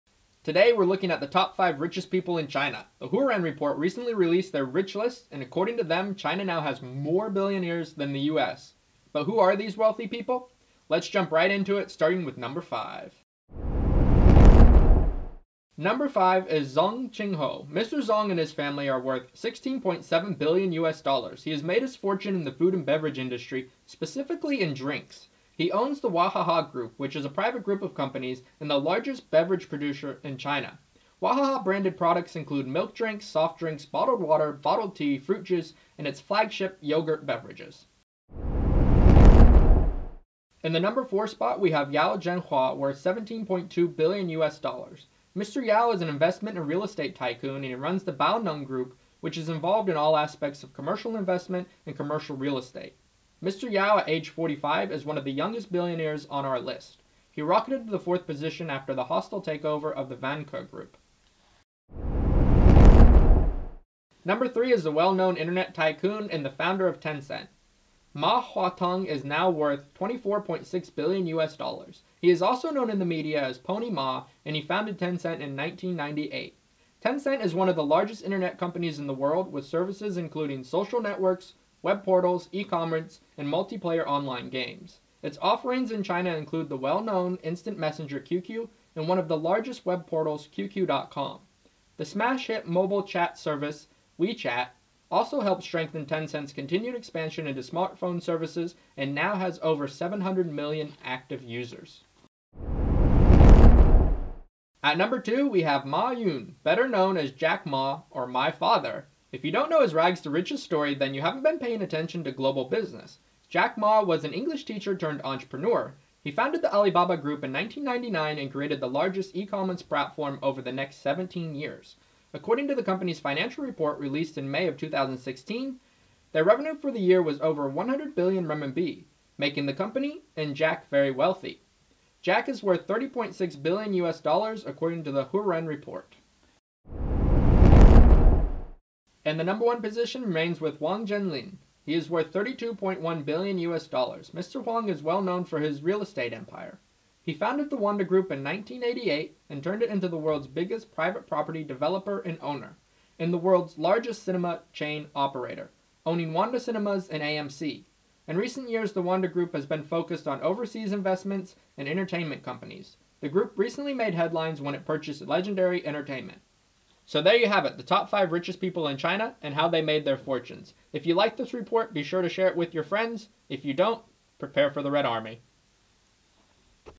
Top 5 richest people in China
Who are the top 5 richest people in China? The Hurun Report recently released their Rich List and according to them, “China now has more billionaires than the US.”

This list counts down the top 5 billionaires in China and how much money they have.